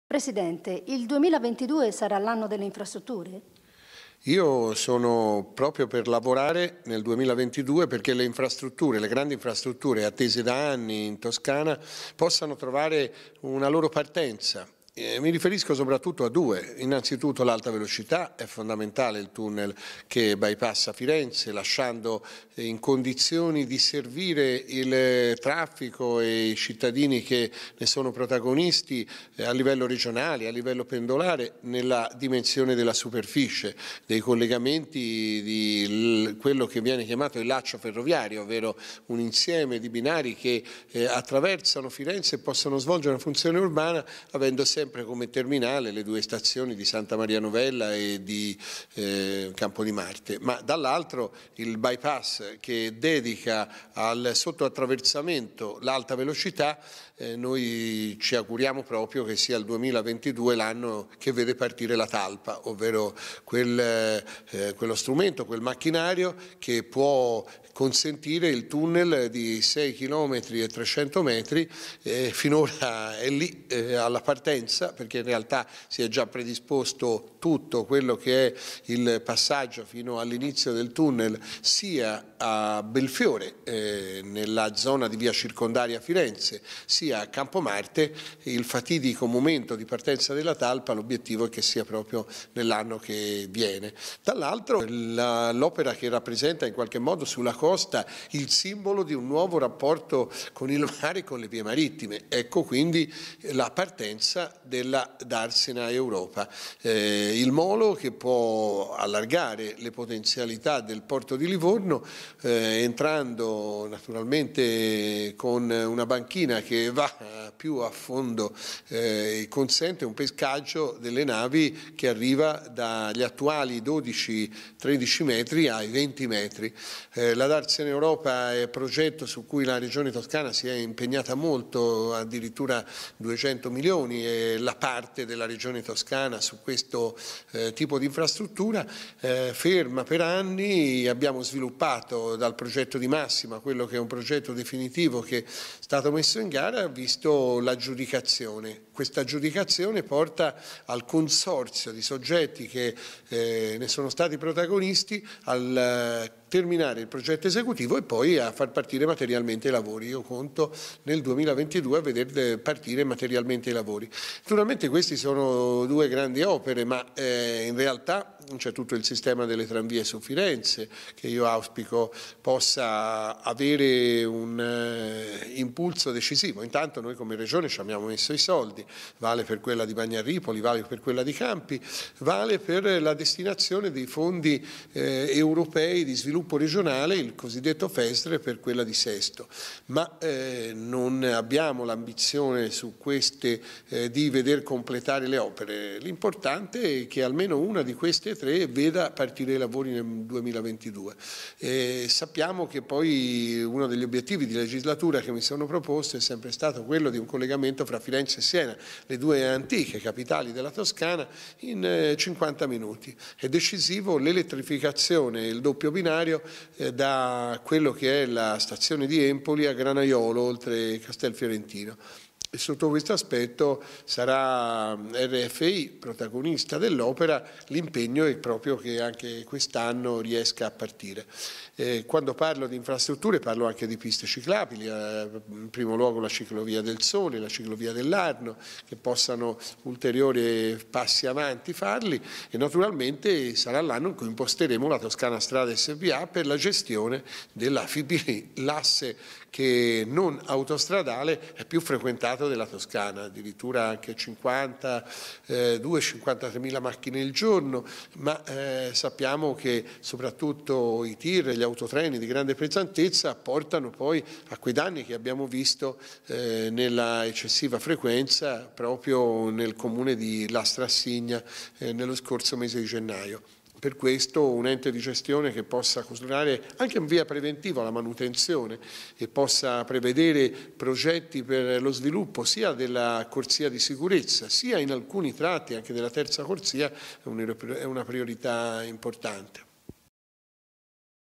Infrastrutture, 2022, l’anno della ripartenza
Il bilancio del primo anno di legislatura del presidente Eugenio Giani e della Giunta con i principali momenti che hanno caratterizzato gli interventi rtegionali in materia di infrastrutture.